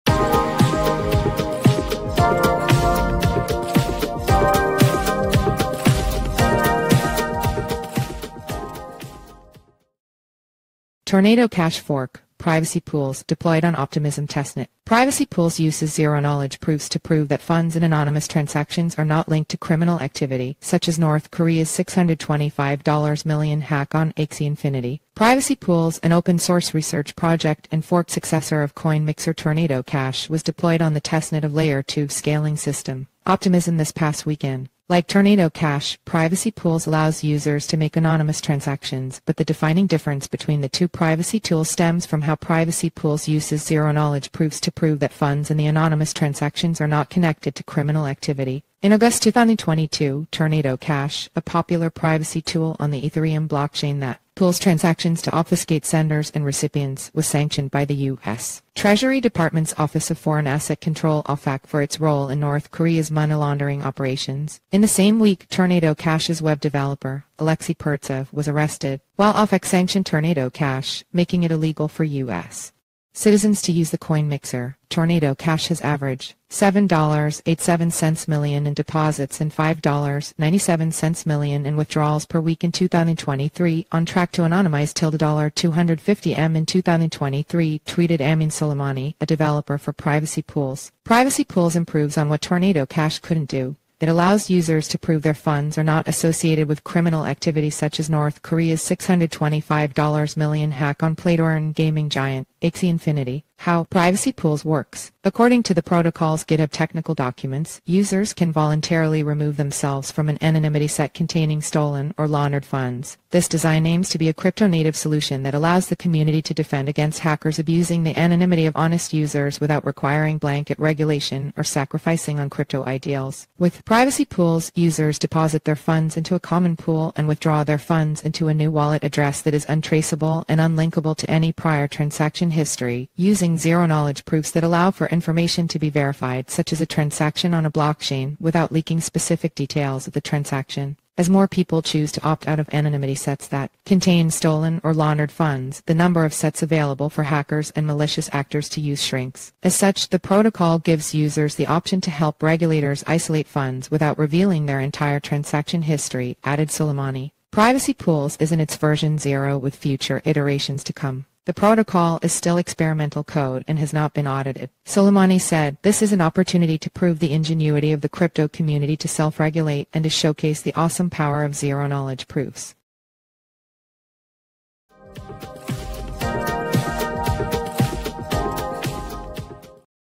Tornado Cash Fork, Privacy Pools, Deployed on Optimism Testnet
Privacy Pools uses zero-knowledge proofs to prove that funds in anonymous transactions are not linked to criminal activity, such as North Korea’s $625 million hack on Axie Infinity.

Privacy Pools, an open-source research project and forked successor of coin mixer Tornado Cash, was deployed on the testnet of layer 2 scaling system Optimism this past weekend.

Like Tornado Cash, Privacy Pools allows users to make anonymous transactions, but the defining difference between the two privacy tools stems from how Privacy Pools uses zero-knowledge proofs to prove that funds in the anonymous transactions are not connected to criminal activity.

In August 2022, Tornado Cash, a popular privacy tool on the Ethereum blockchain that pools transactions to obfuscate senders and recipients, was sanctioned by the U.S. Treasury Department’s Office of Foreign Asset Control (OFAC) for its role in North Korea’s money-laundering operations. In the same week, Tornado Cash’s web developer, Alexey Pertsev, was arrested.

While OFAC sanctioned Tornado Cash, making it illegal for U.S. citizens to use the coin mixer, Tornado Cash has averaged $7.87 million in deposits and $5.97 million in withdrawals per week in 2023, “on track to anonymize ~$250M in 2023,” tweeted Ameen Soleimani, a developer for Privacy Pools.

Privacy Pools improves on what Tornado Cash couldn’t do: It allows users to prove their funds are not associated with criminal activity such as North Korea’s $625 million hack on play-to-earn gaming giant, Axie Infinity.

How Privacy Pools works.

According to the protocol’s Github technical documents, “Users can voluntarily remove themselves from an anonymity set containing stolen or laundered funds … This design aims to be a crypto-native solution that allows the community to defend against hackers abusing the anonymity of honest users without requiring blanket regulation or sacrificing on crypto ideals.”

With Privacy Pools, users deposit their funds into a common pool and withdraw their funds into a new wallet address that is untraceable and unlinkable to any prior transaction history, using zero-knowledge proofs that allow for information to be verified, such as a transaction on a blockchain, without leaking specific details of the transaction.

As more people choose to opt out of anonymity sets that contain stolen or laundered funds, the number of sets available for hackers and malicious actors to use shrinks. As such, the protocol gives users “the option to help regulators isolate funds, without revealing their entire transaction history,” added Soleimani.

Privacy Pools is in its “version zero” with future iterations to come. The protocol is still experimental code and has not been audited. Soleimani said, “This is an opportunity to prove the ingenuity of the crypto community to self-regulate and to showcase the awesome power of zero-knowledge proofs.”